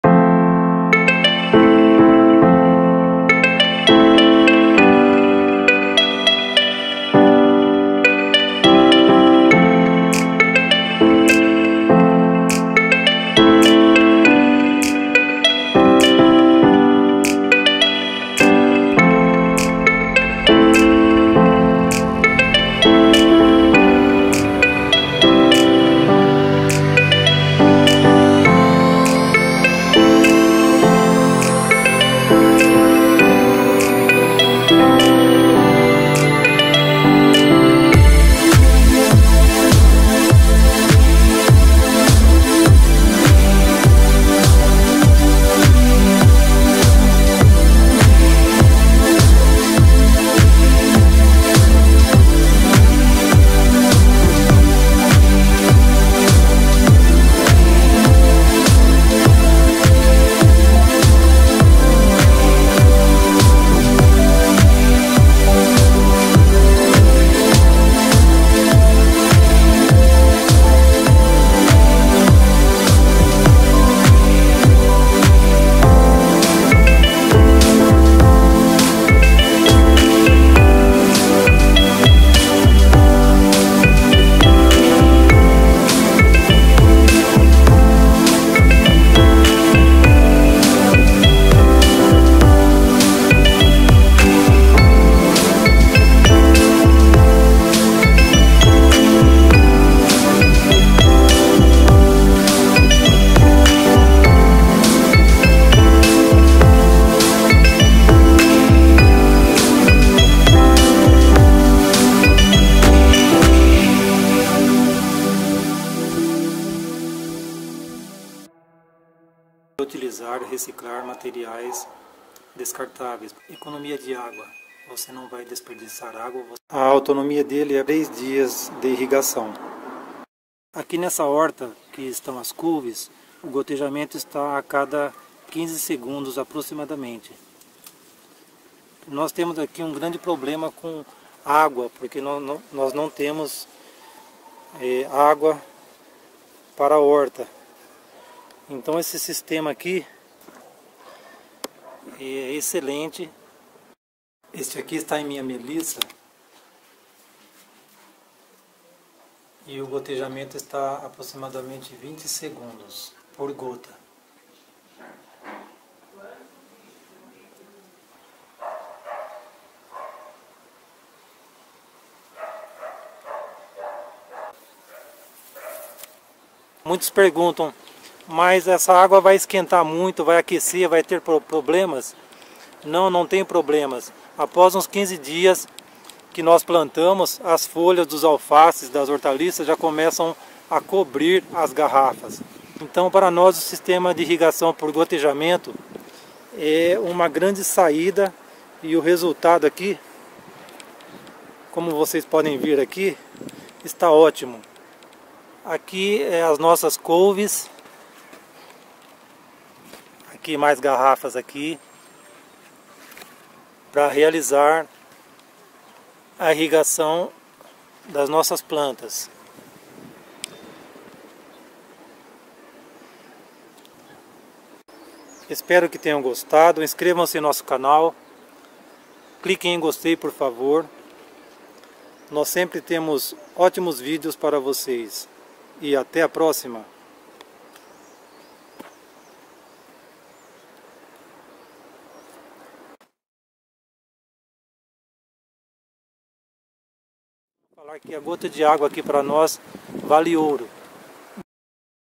How to make Self Watering System for your Plants using waste Plastic Bottle and a Rope 2
You will learn How to make Self Watering System for your Plants using waste Plastic Bottle and a Rope 2.
Sustainable and Organic Vegetable Garden After 45 days we planted the development of the Plants result is excellent, irrigation is performed by Capillarity.
Ropes transfer water by dripping
There are no heating problems since after about 15 days the leaves are already covering the Lettuces Plants
This Irrigation system has several advantages:
1- We are recycling and reusing Disposable Materials
2- Economics of water, no waste
3- Irrigation Autonomy for 3 days,
Let's start!
With the Welding Iron I will make holes to house the Barbantes
I cut the Strings with 25 cm, and I will put in the holes
With the Scissors I will cut in X
To the hose inlet for water replenishment
The purpose is for when removing the hose the hole is completely closed, this prevents the entry of insects, for example mosquitoes.
These Bottles are in our Garden with Cabbages, the drip is at 15 seconds, we have problems with lack of water, so it is an alternative to Irrigation and water saving.
Thank you friends to view the Movie
 click on please liked,
welcome!
Adriano.